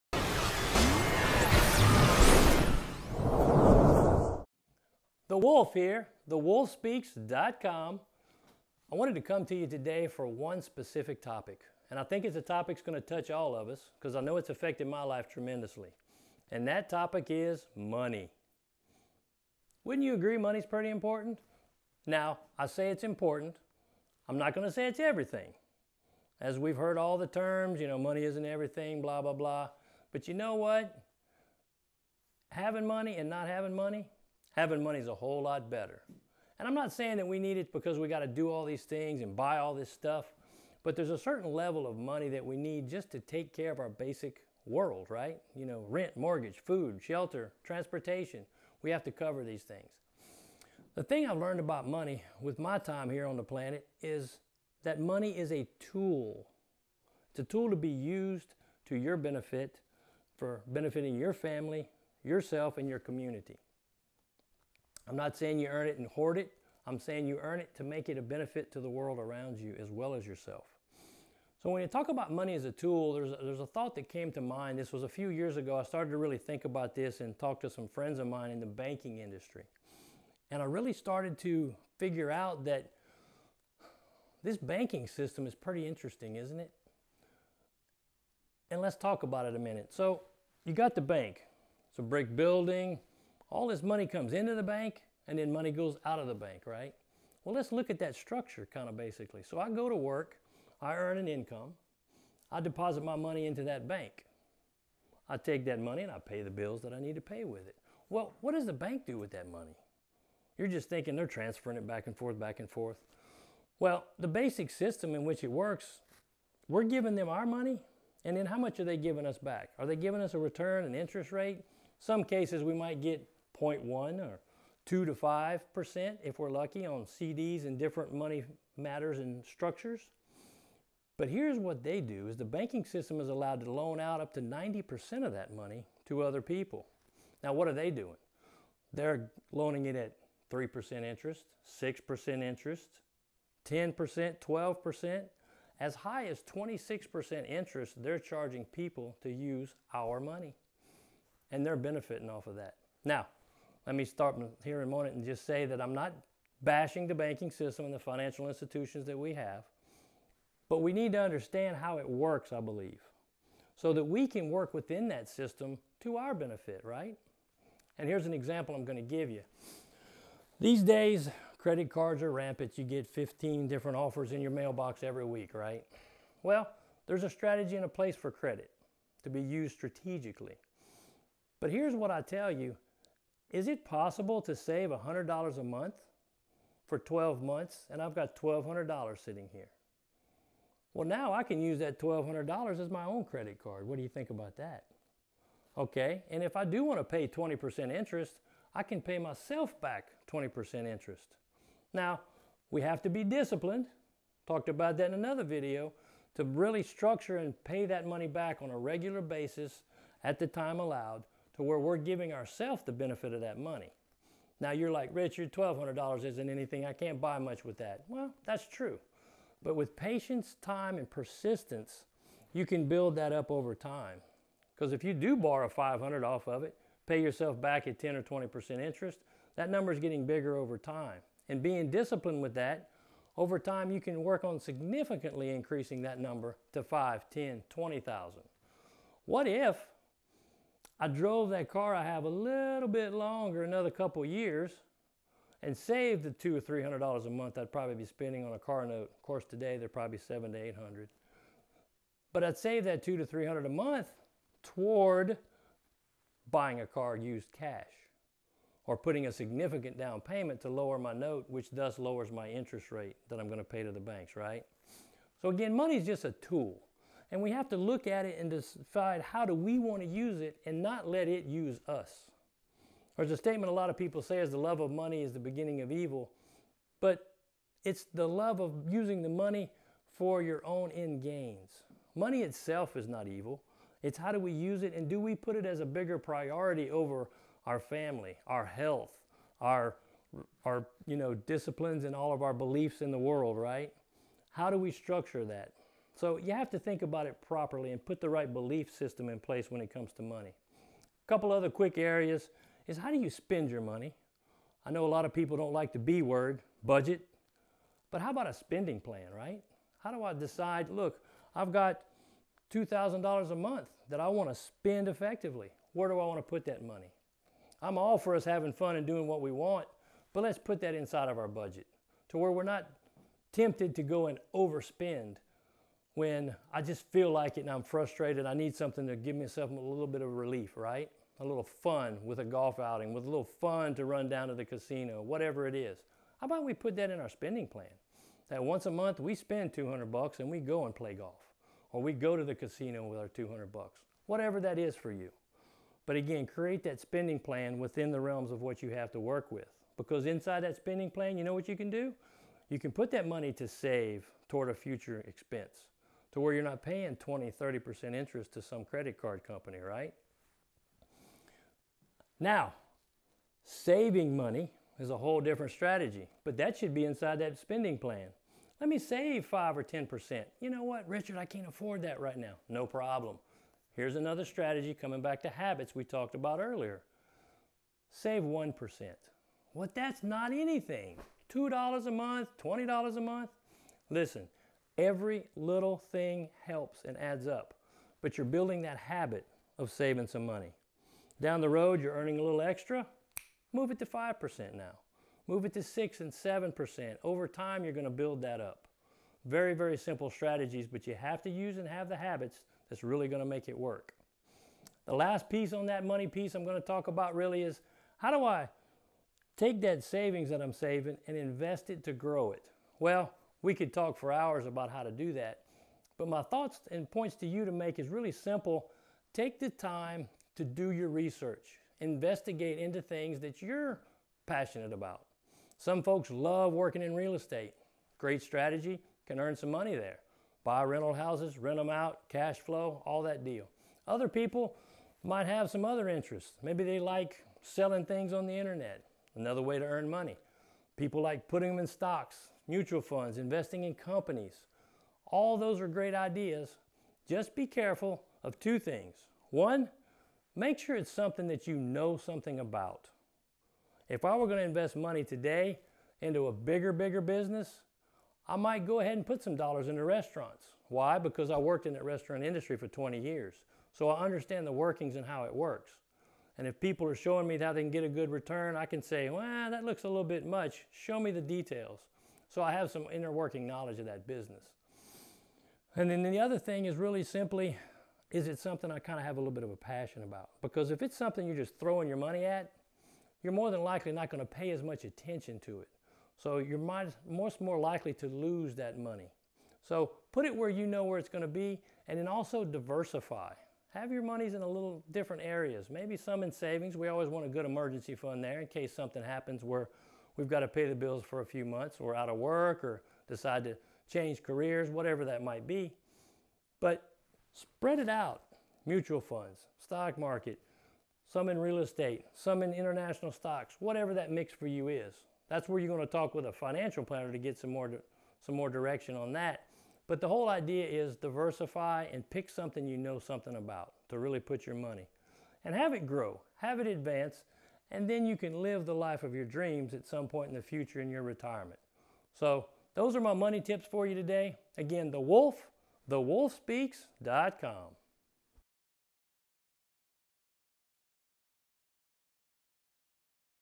Money is a Tool nothing more
As I look back over my life Mindset, Money and Marriage are the 3 areas I would like to have known more about.